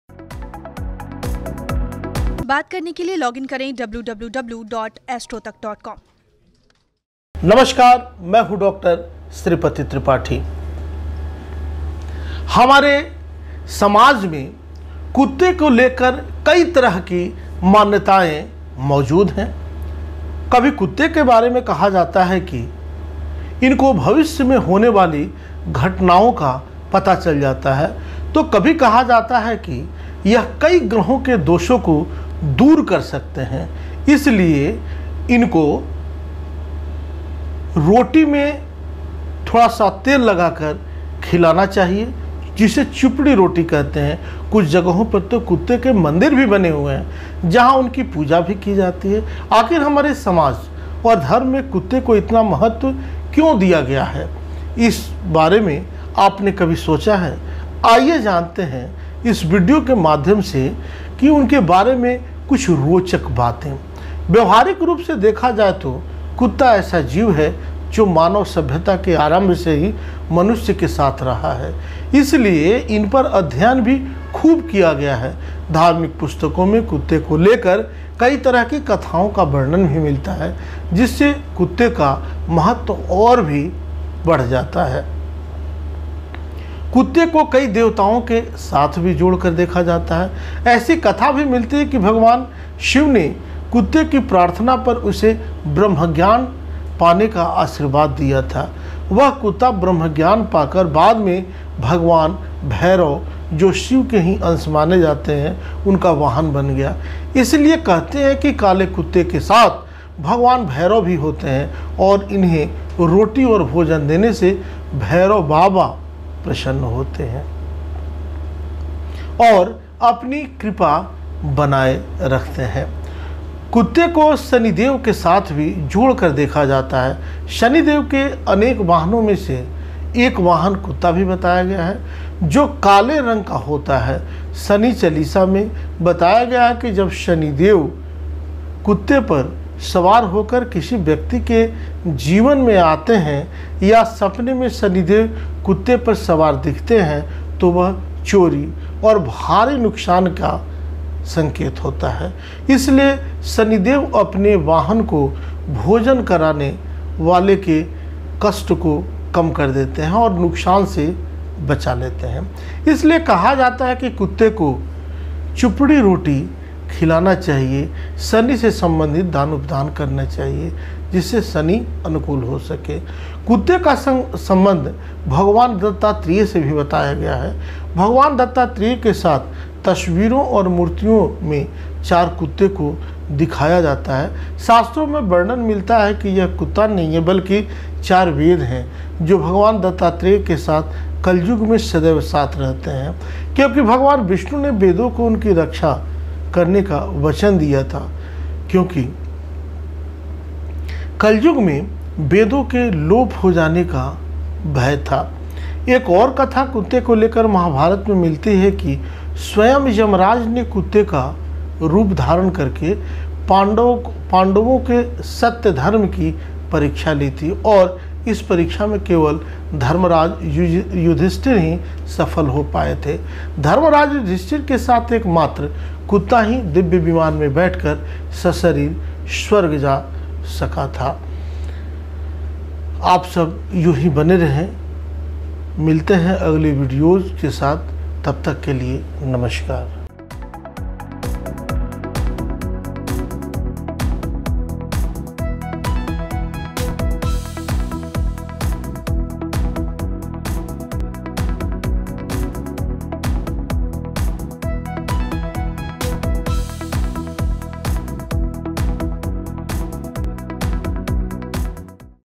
कुत्ते को रोटी खिलाने से क्या फायदा होता है | Dr Sripati Tripathi| Astro Tak
ज्योतिर्विद डॉ. श्रीपति त्रिपाठी जी से जानते हैं कि कुत्ते को रोटी खिलाने से क्या फायदा होता है...साथ में ये भी जानते हैं कि कुत्ता धार्मिक दृष्टि से क्यों खास है...